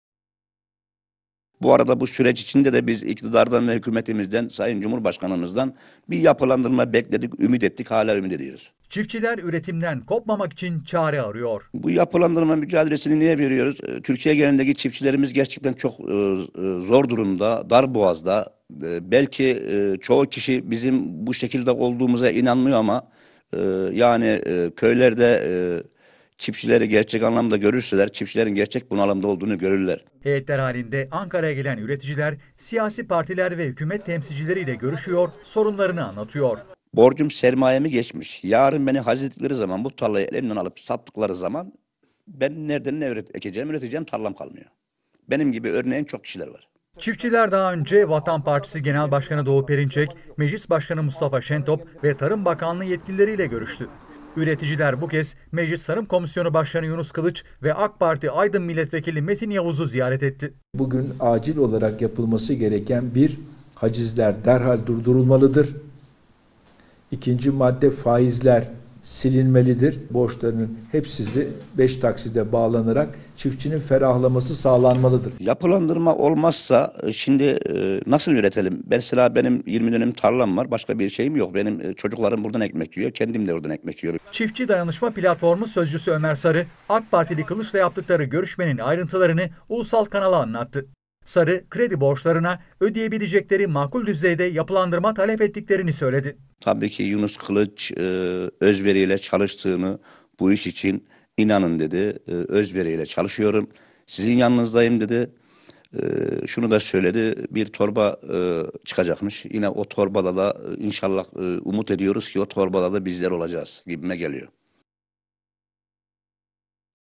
Borçlarını ödeyemeyen çiftçiler çözüm için Ankara'ya gitti
Borçlarını ödeyemedikleri haciz terörü ile karşı karşıya olan üretici çözüm için yine Ankara'ya gitti. Bu kez adresleri Meclis Tarım Komisyonu Başkanı Yunus Kılıç oldu. Görüşmede, kredi krizinin aşılması için çözüm yolları ele alındı.